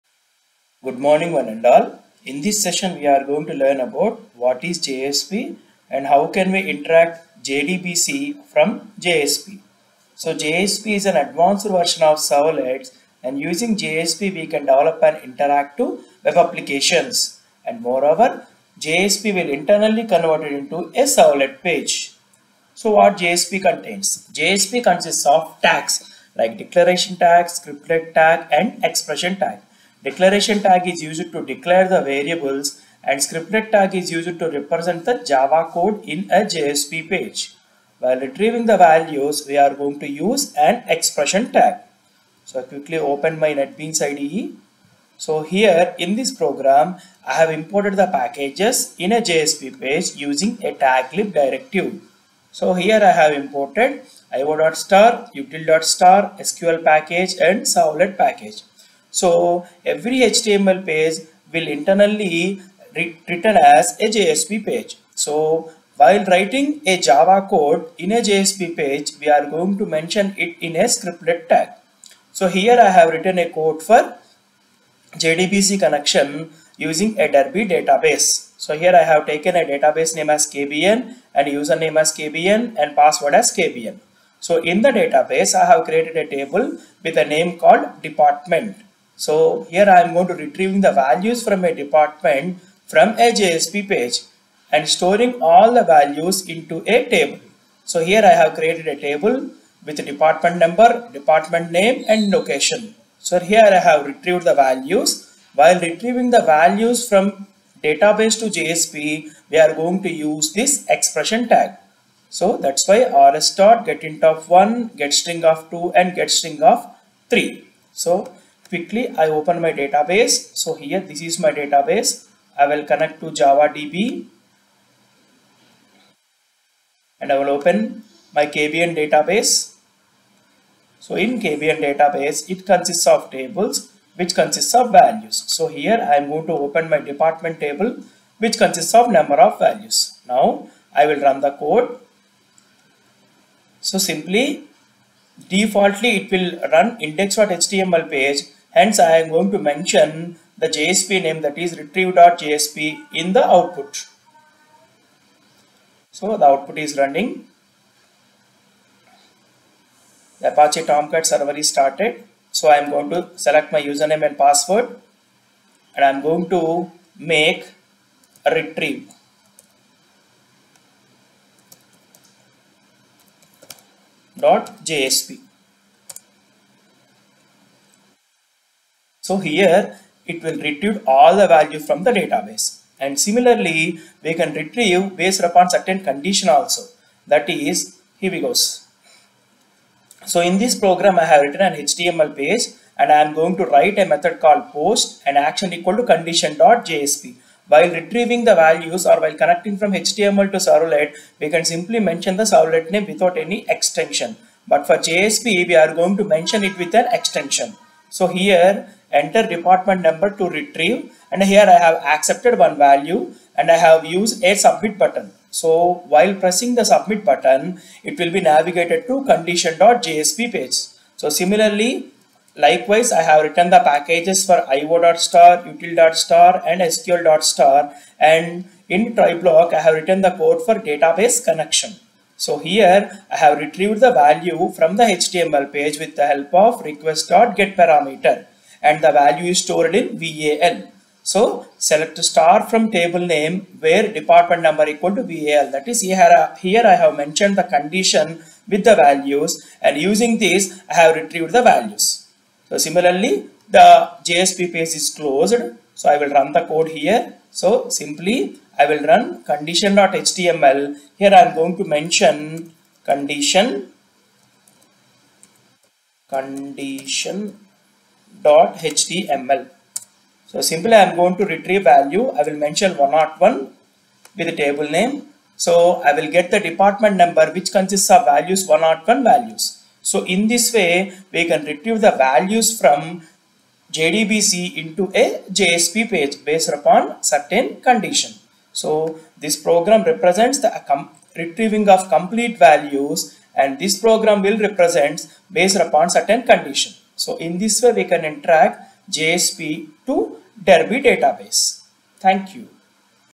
Jsp and Interaction with Derby Database
What is Jsp , How can we interact to JDBC from a JSP Page with the help of tags and how can we retrieve the values based on Condition